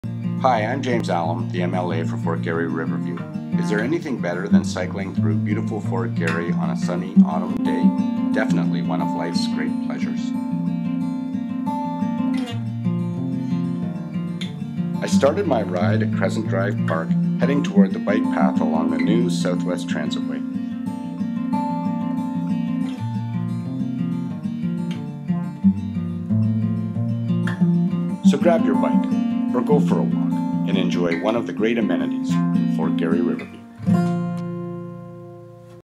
Fort Garry-Riverview Biking & Hiking Trail
As the MLA for the Fort Garry-Riverview area, I'm happy to present a short video to showcase opportunities for hiking and cycling in Fort Garry-Riverview. Enjoy!
(Directed and Edited by Kevin Tabachnick, Composed by James Allum and Kevin Tabachnick, Music by Nicholas Van Doeselaar)